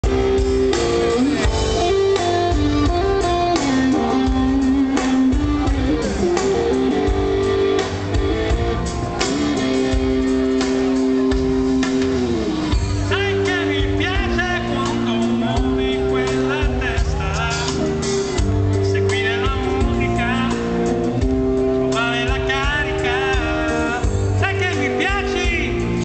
Pino Daniele in concerto a Reggio Calabria
Concerto di Pino Dainele a Reggio Calabria